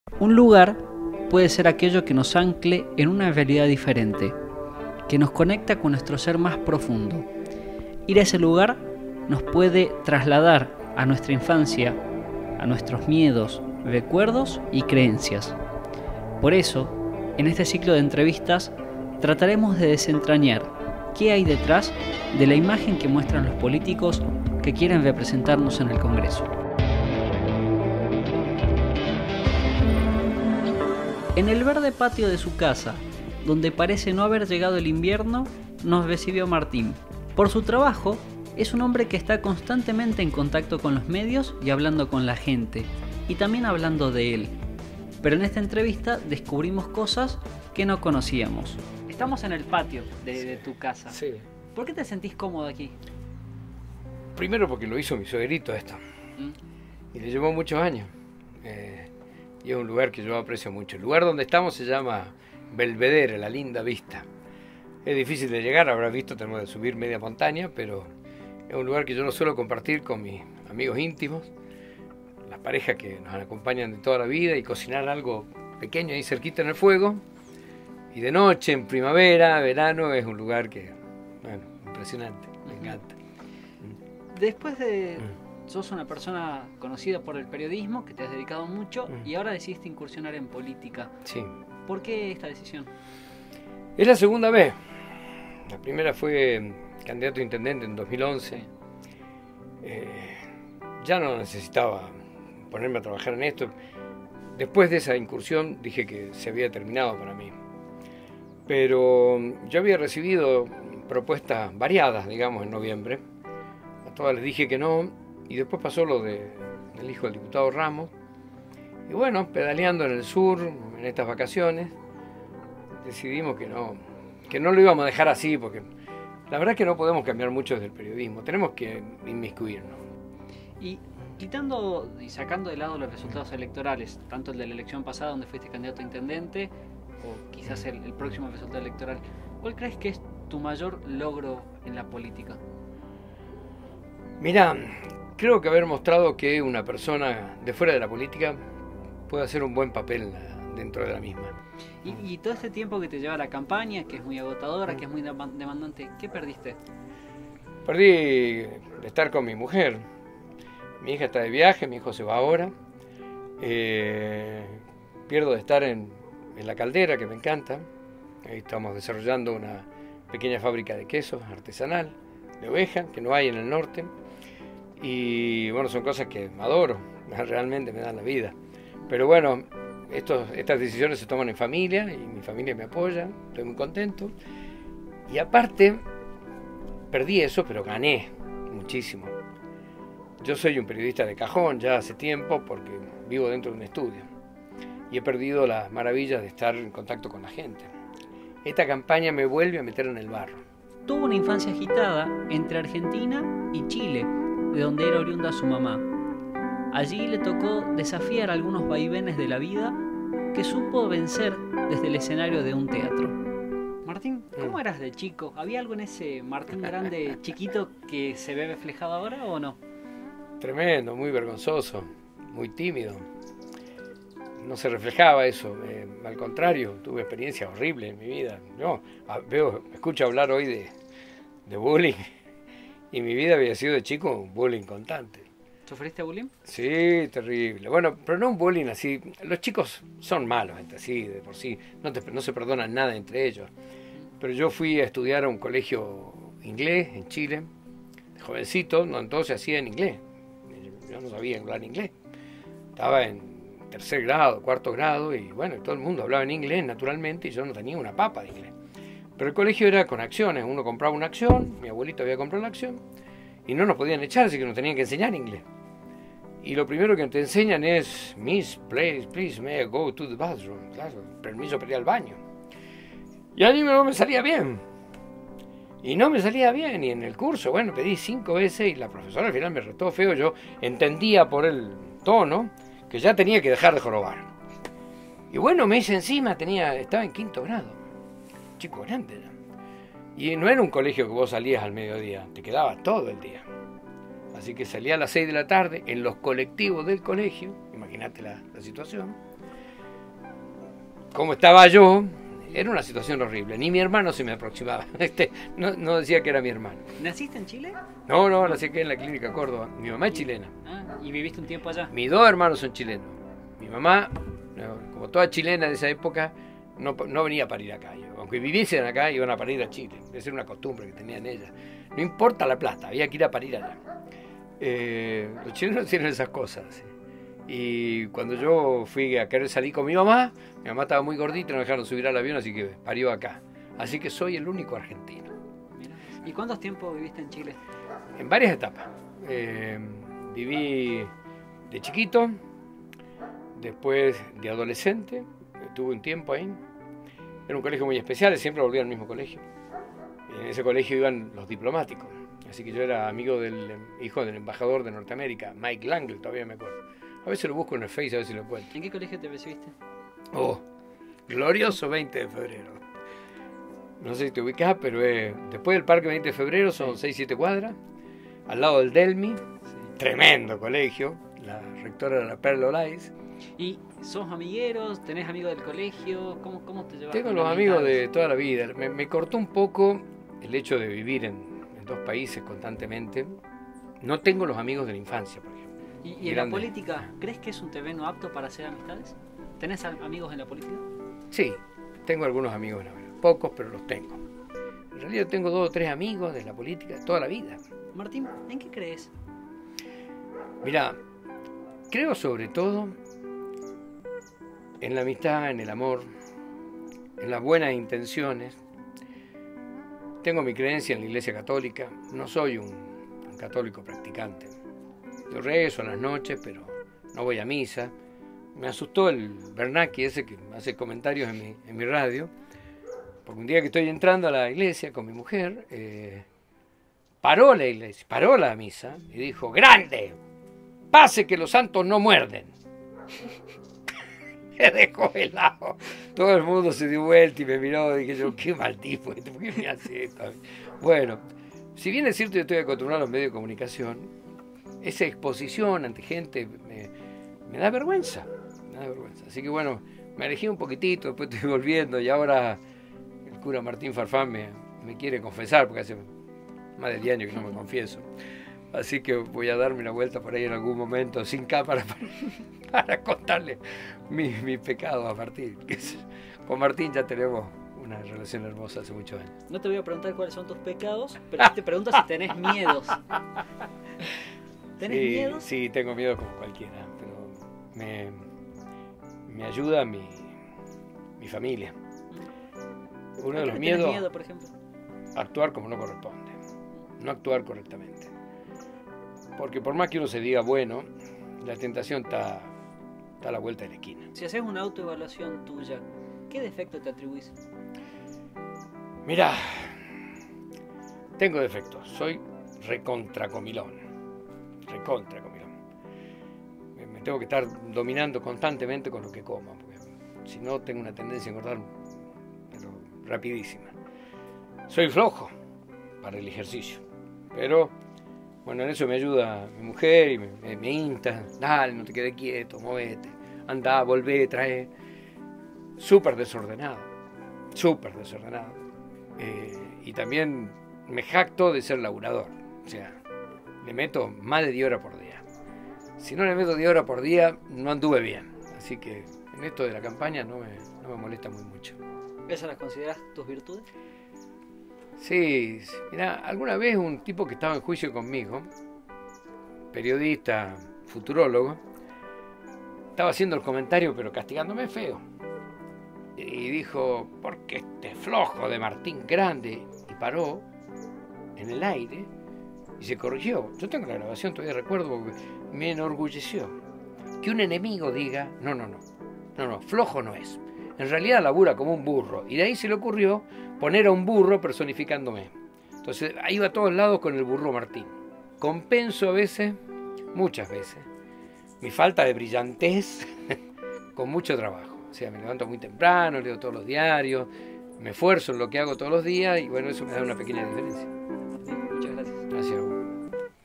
MI LUGAR: ¿Quién es Martín Grande?
En este ciclo de entrevistas a los pre candidatos legislativos te contamos detalles de la vida del periodista radial que vivió mucho tiempo en Chile, sufrió bullying, y salió adelante gracias al teatro.